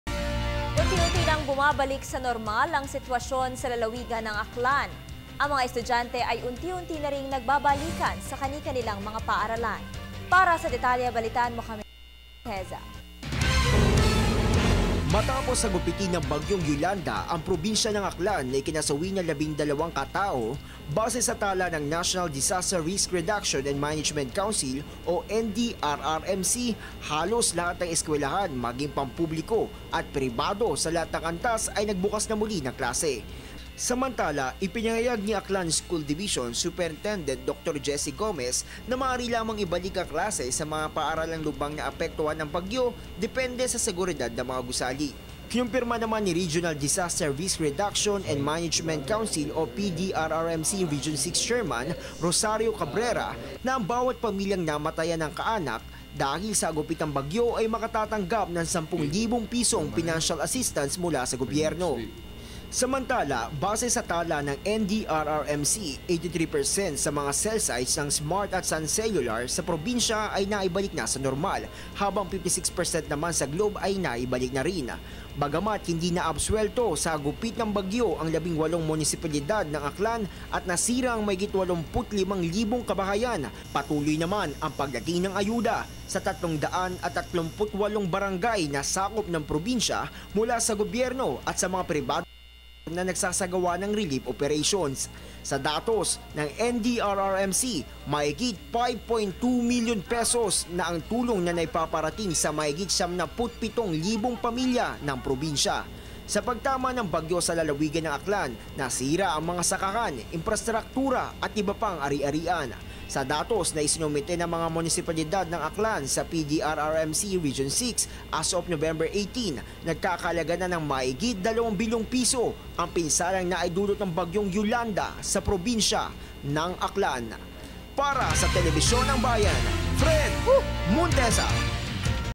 Balitaan: Lalawigan ng Aklan, unti-unti nang nakakabangon mula sa pinsala ng bagyong Yolanda
Balitaan: Lalawigan ng Aklan, unti-unti nang nakakabangon mula sa pinsala ng bagyong Yolanda (Ulat ni Fredd Monteza) [November 20, 2013]